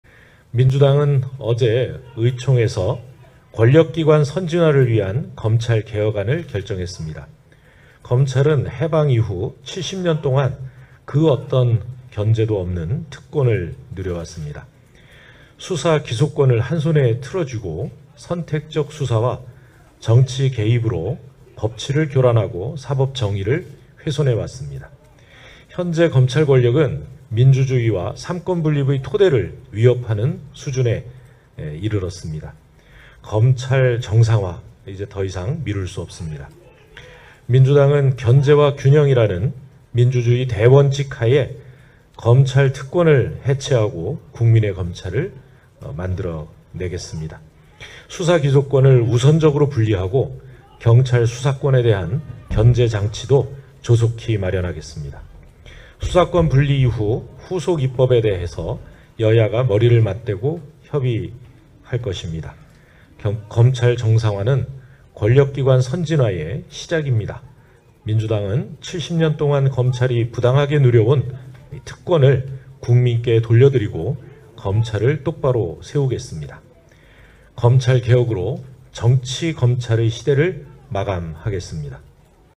윤호중 비대위원장 "검찰 정상화는 ‘권력기관 선진화’의 시작"
제15차 비상대책위원회의 모두발언

2022년 4월 13일(수) 오전 11시 더불어민주당 대전시당 대회의실